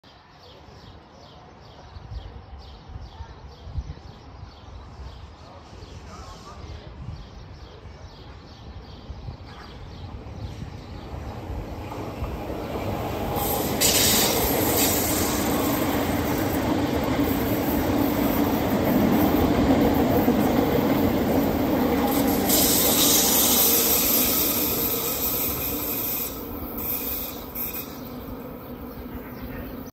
Rhätische Bahn RHB RegioExpress Landquart - St. Moritz
der RegioExpress der Rhätischen Bahn (RHB) ist gerade in Landquart losgefahren um knapp 2 Stunden später in St. Moritz anzukommen.